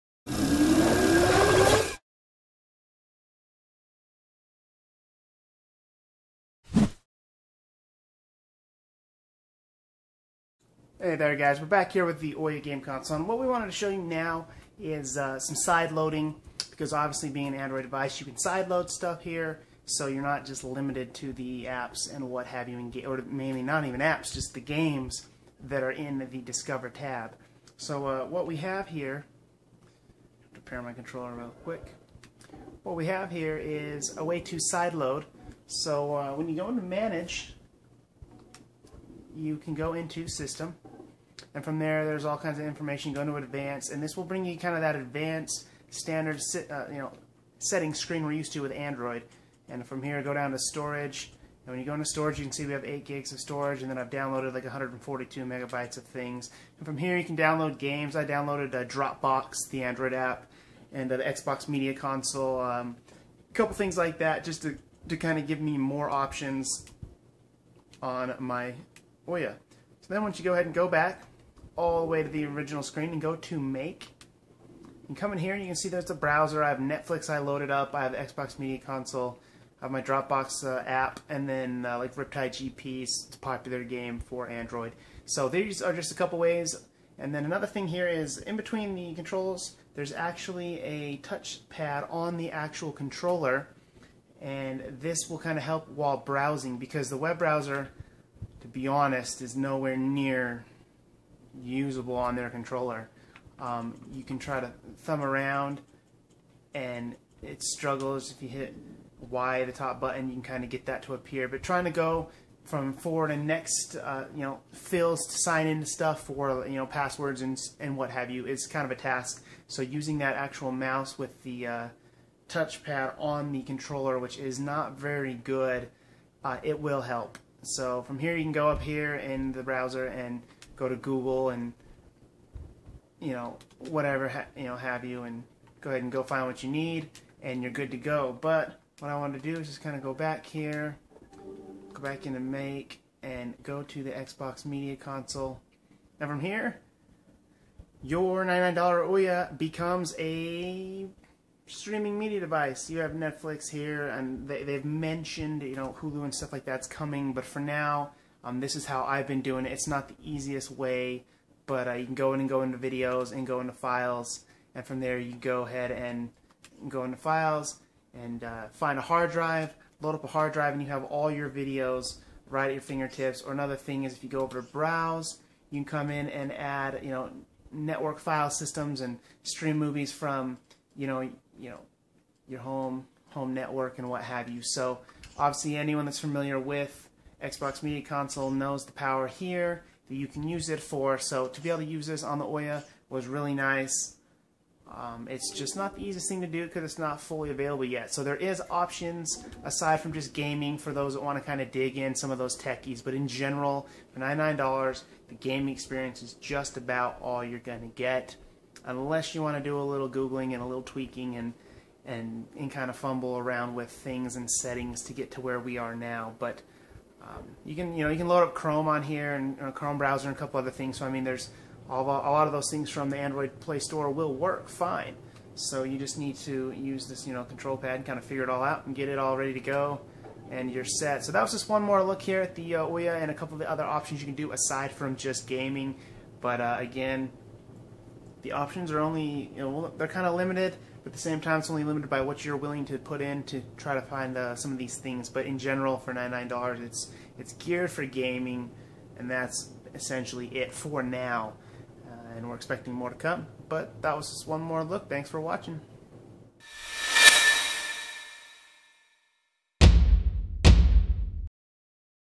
OUYA Console app side-loading and XBMC
Side-loading apps like XBMC on the OUYA Game Console, which makes it a living room media streamer too. More at Androidcommunity.com